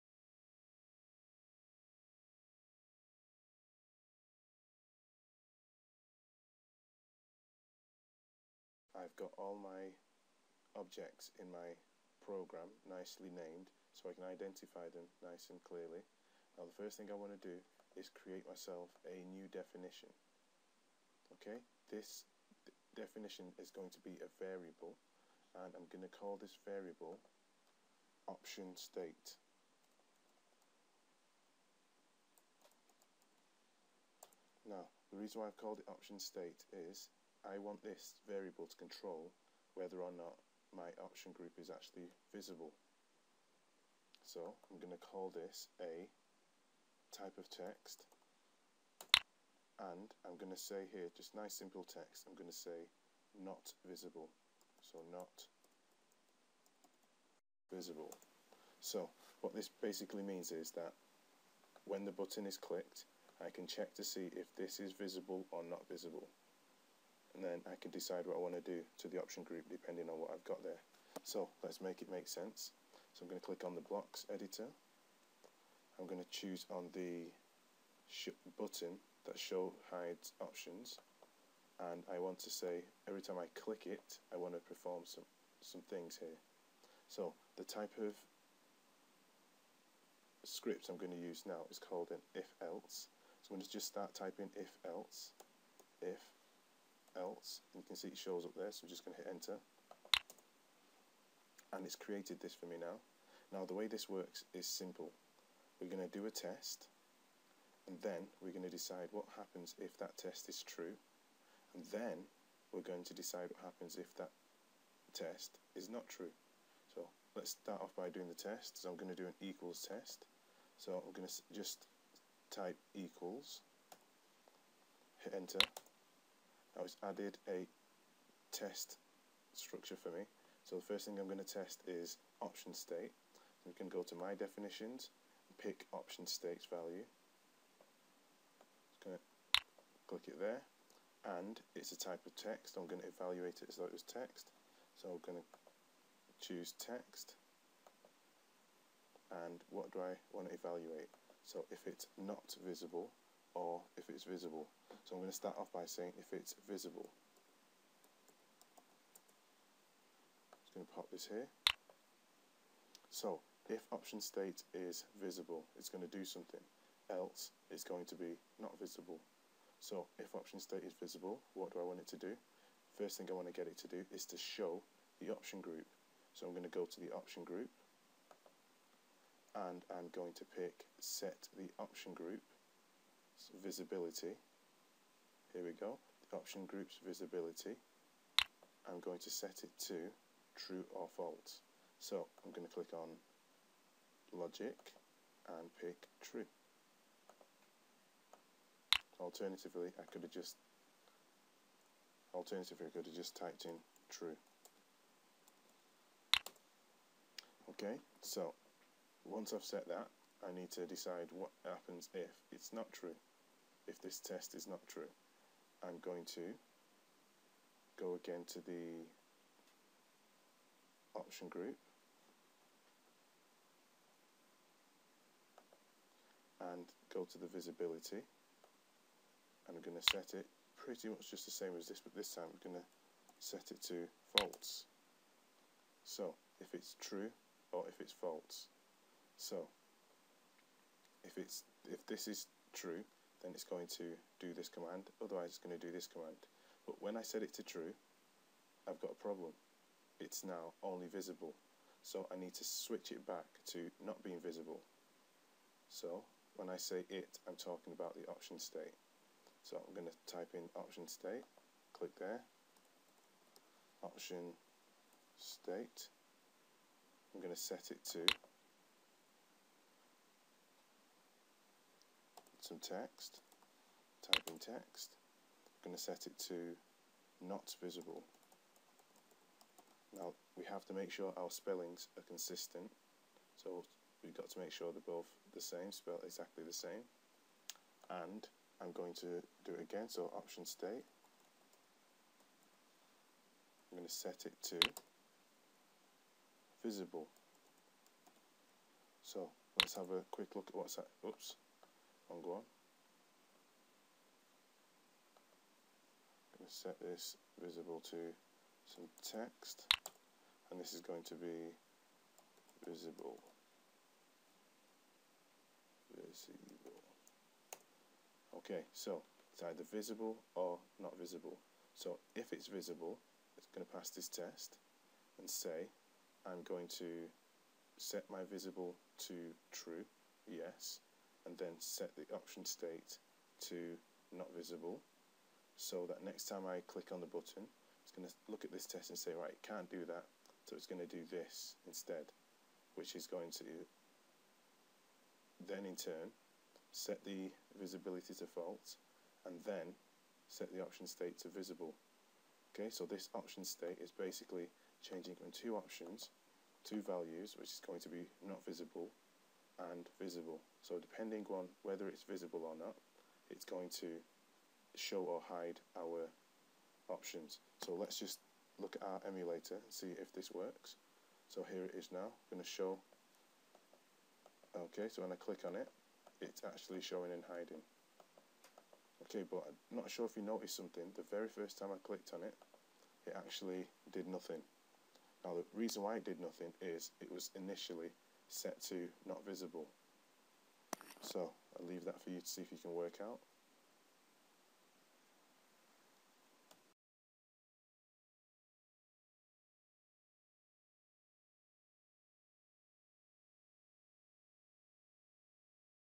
App Inventor - How to create a Toggle Button
In this short lesson, I show you how to create a toggle button, the toggle can work between two, three or as many different states as you like.